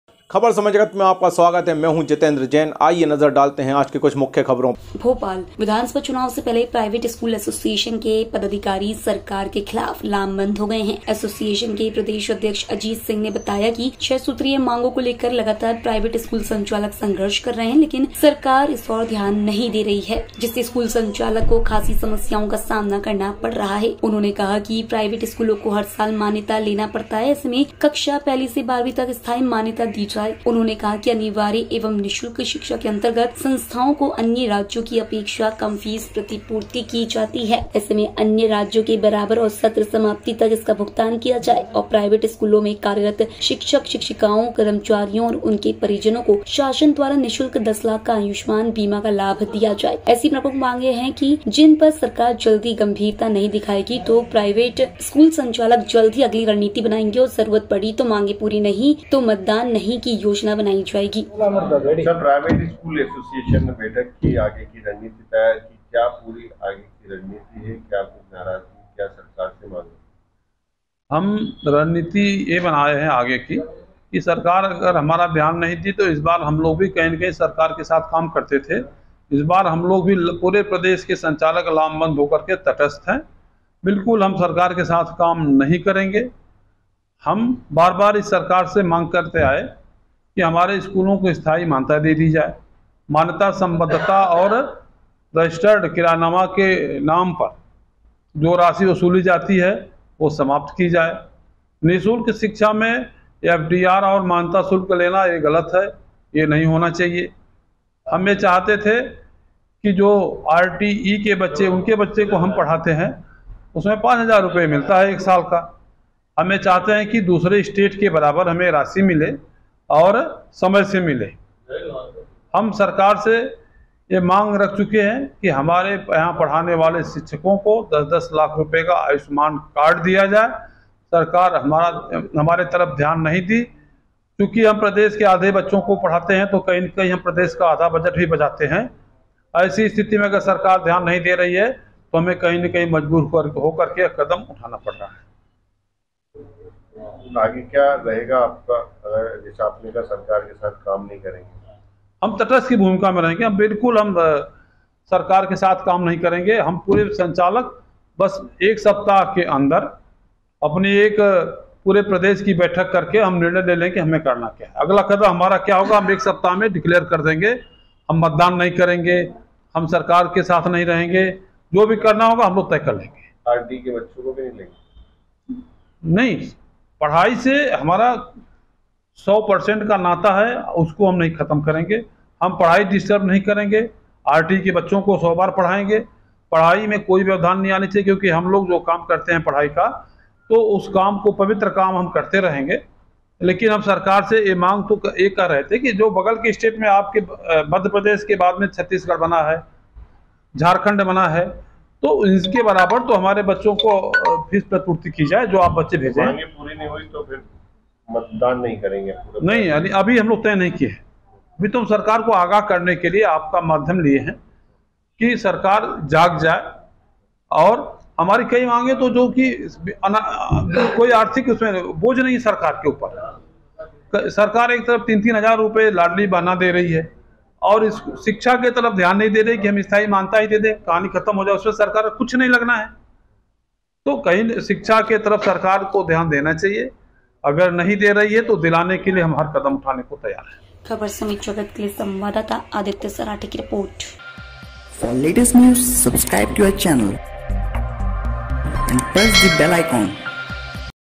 भोपाल:- प्राइवेट स्कूल संचालकों की चेतावनी,6 सूत्रीय मांगें जल्द पूरे करे सरकार !!!
आदित्य सराठे